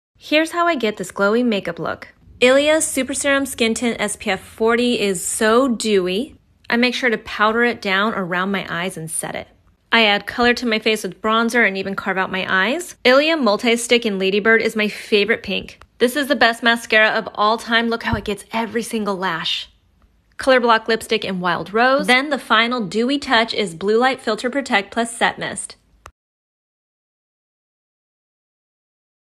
Easy & Natural Everyday Makeup
Glowy, natural-looking makeup is my vibe! Here’s what I used from ILIA Beauty:

Super Serum Skin Tint SPF 40 (in Matira). It’s dewy, has niacinamide, squalane, and hyaluronic acid, and mineral filters.

Soft Focus Finishing Powder to set around the eyes and where needed.

Nightlite Bronzer Powder to add color and warm up my skin

Multi-Stick (in Lady Bird) has been a go-to blush for years!

Limitless Lash Mascara is my ALL-TIME FAVORITE due to the amazing brush. I always have a tube of it.

Then the important final step: Blue Light Filter Protect + Set Mist to bring back the overall glow, set my makeup, and give some added pollution and blue light protection (from the sun, not my phone! 😛).

What are your faves from ILIA Beauty?